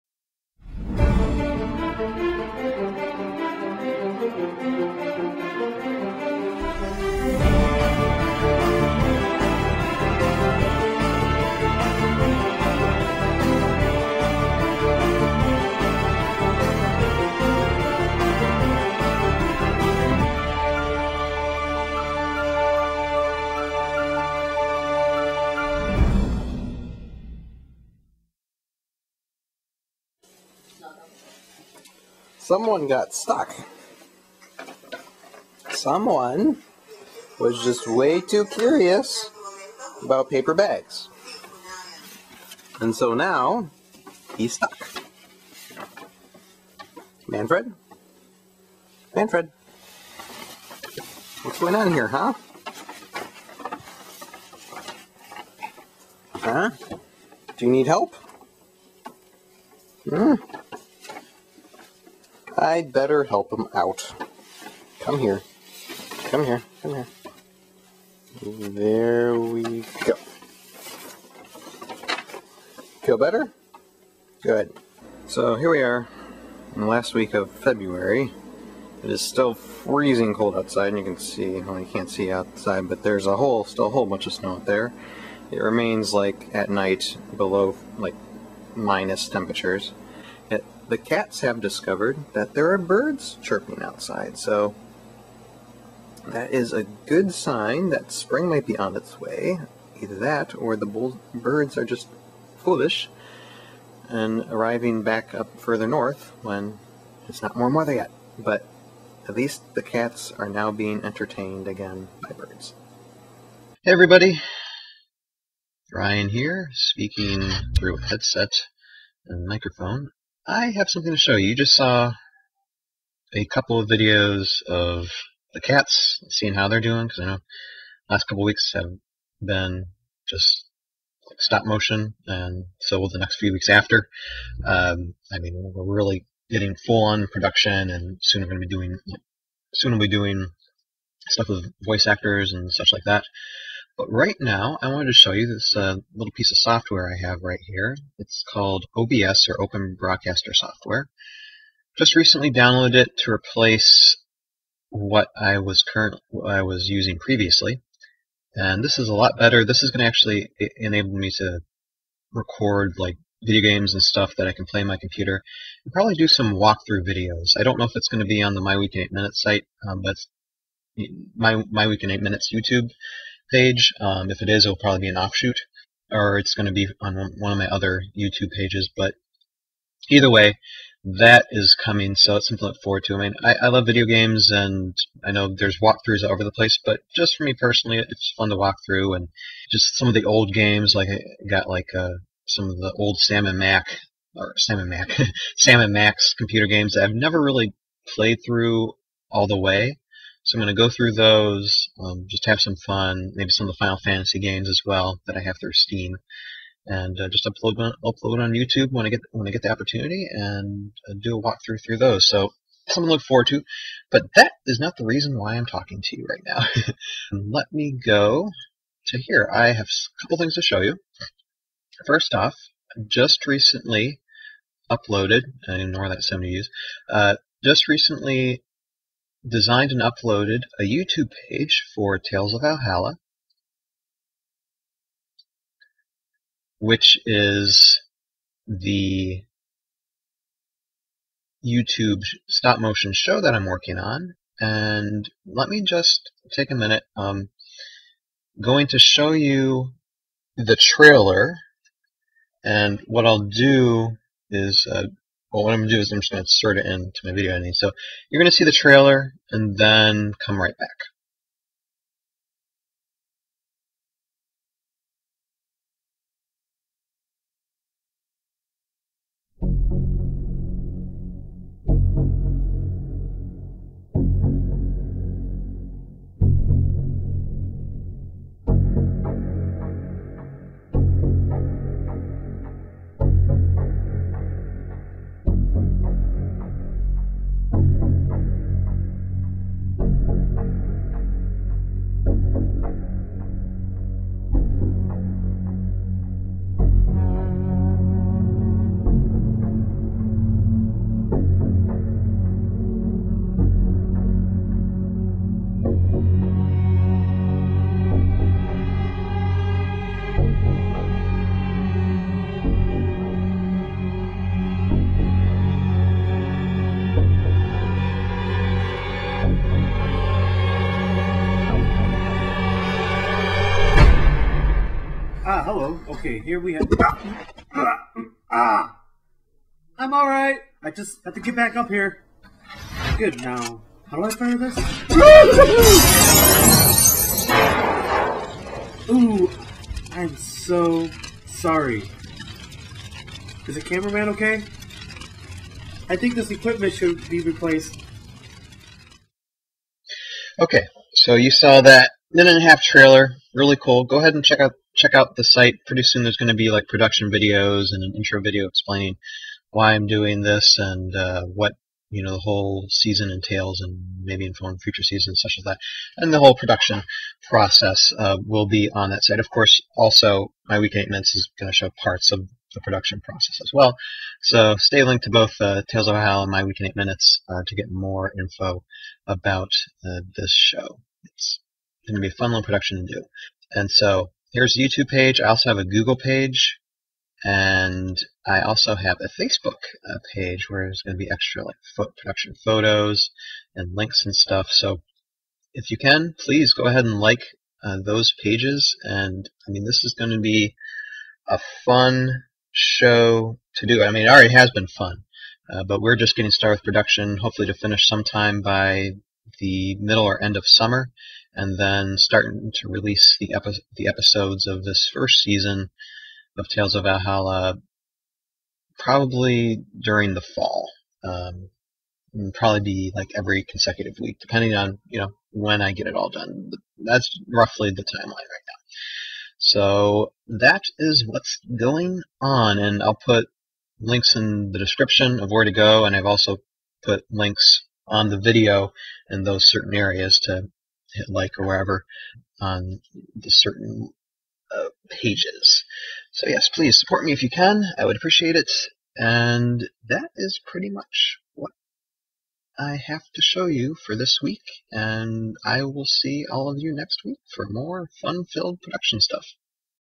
A Teaser Trailer! (Week 8 2015)
This Week 8 2015 I check on the cats and see what they are doing, I get on my computer and show you the teaser trailer for Tales of Valhalla; The Aftermath Chronicles Season 1 and the websites for the show as well and I talk about a couple other things that I have planned.